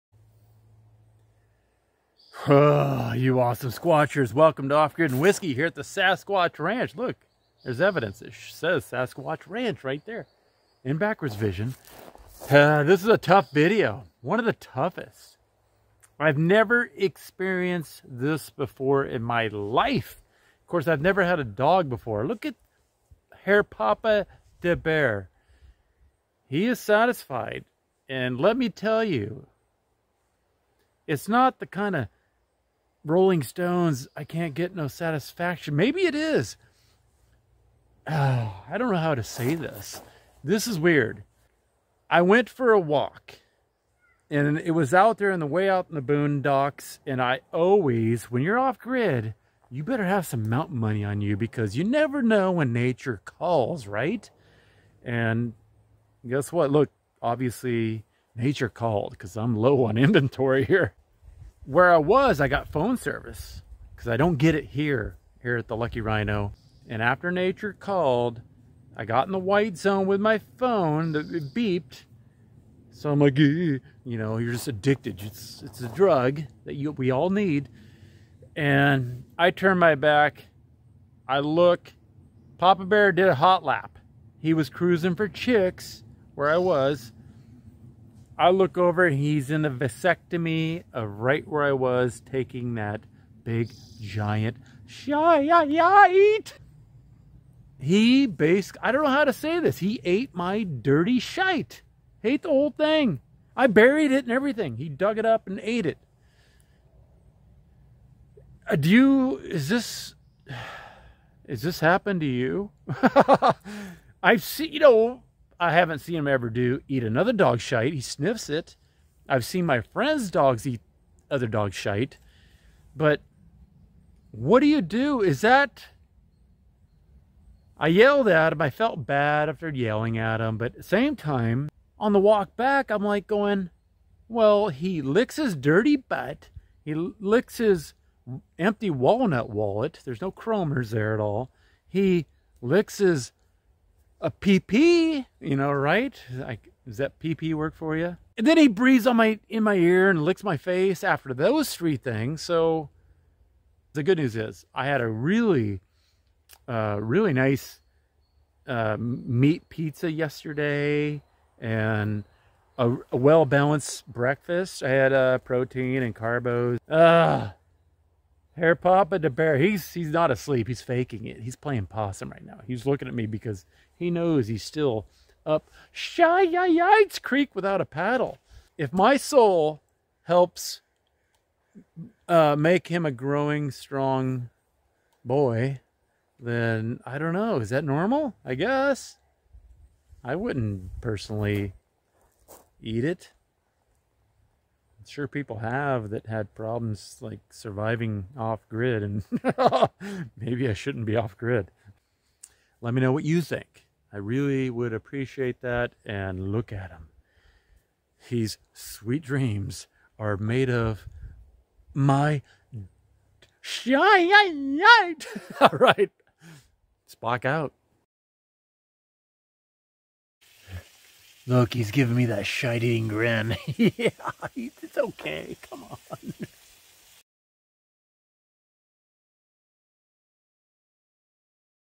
The Off-Grid Doberman Diet
Well balanced diet is the cornerstone to a healthy pooch.

Thank you all for supporting the Poop for a Pooch Foundation!